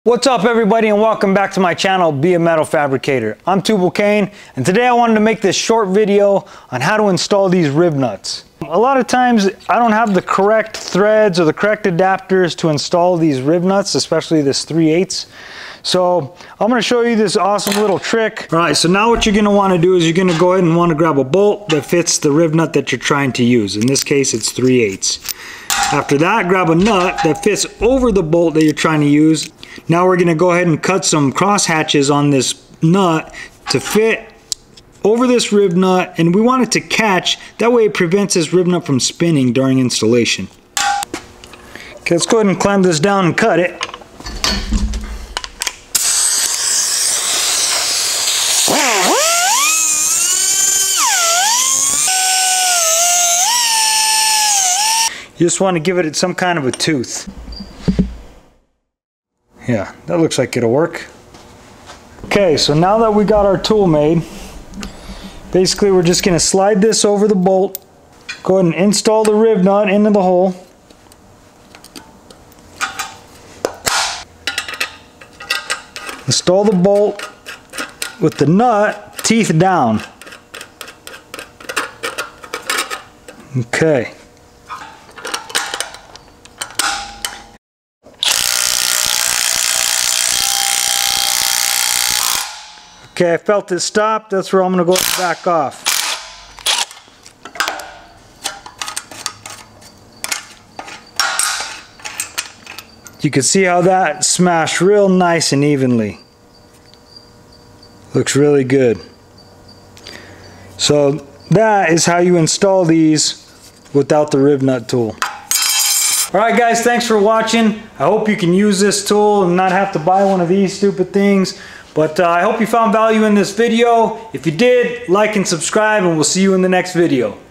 DIY Rivnut tool- Homemade RivNut /Nutsert tool- easy to make!
This is a short video on how to make a RivNut tool. DIY Rivnut tool, or Homemade nutsert tool.

Cleco Fastener Kit
Makita 16 Gauge Shear
Cut off wheel
36 grit Roloc
Strip disk